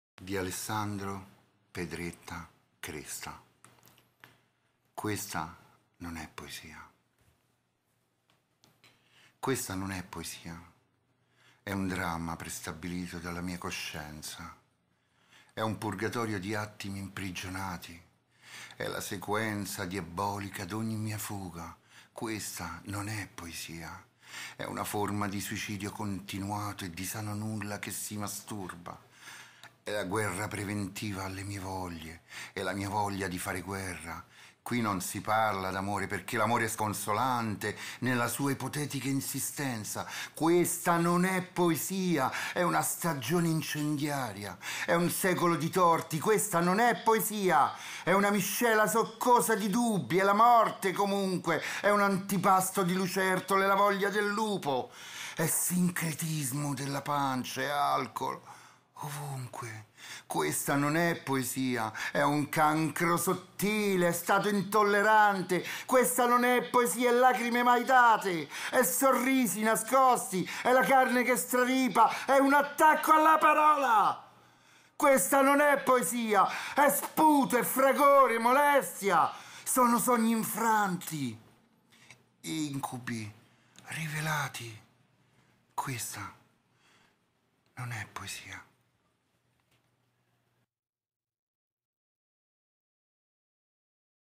Enrico Marra Questa non è poesia di Alessandro Pedretta Kresta
Questa non è poesia.
E' un dramma prestabilito dalla mia coscienza.
E' un purgatorio di attimi imprigionati.
E' la sequenza diabolica di ogni mia fuga.